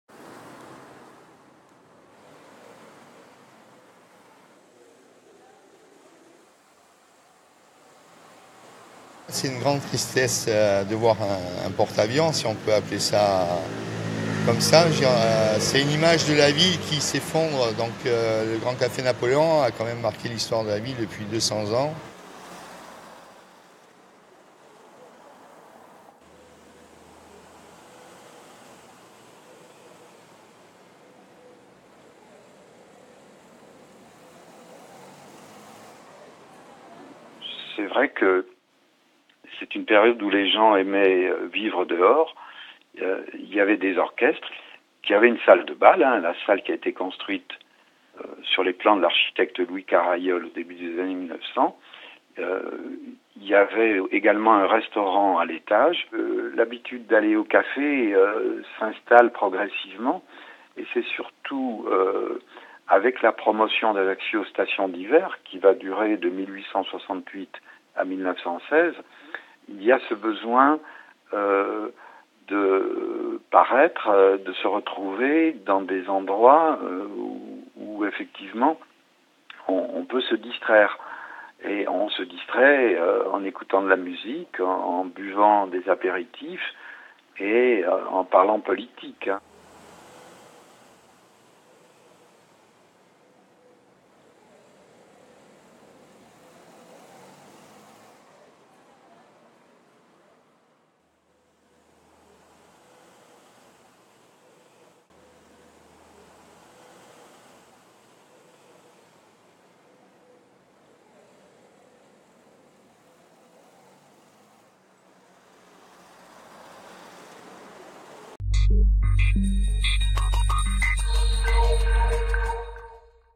Histoire du Café Napoléon
Intervenants -Pascal Bruno
Cafetier

Equipe -
CRISTIANI Marie
STEFANI Jacques-Paul